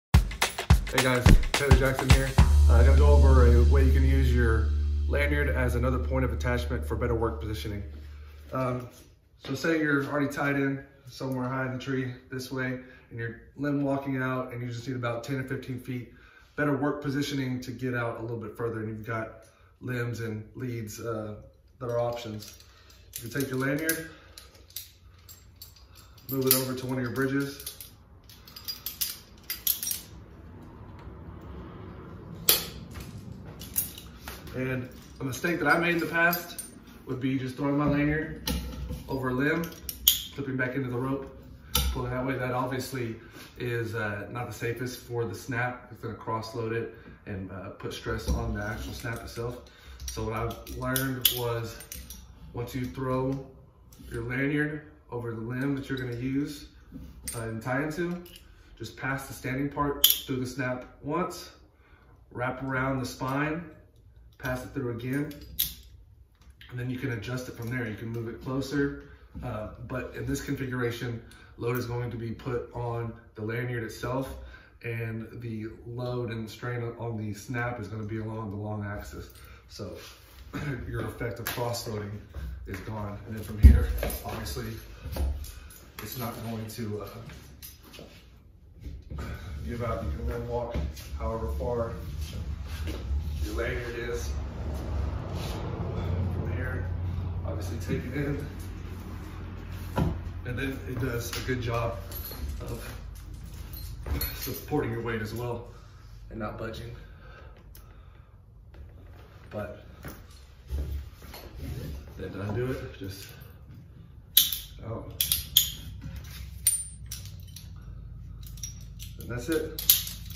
Using your Lanyard for Work Positioning with Taylor Jackson - TreeStuff Community Expert Video
Ever been trying to get to a work position, but just haven't been able to get to the right spot? Taylor Jackson has a tip for you!

Find this tip helpful? Give the video a 'thumbs up' so other arborists will see it!

If you'd like to share your thoughts with TreeStuff and make some money,

A special thanks to Weaver Arborist for making this possible!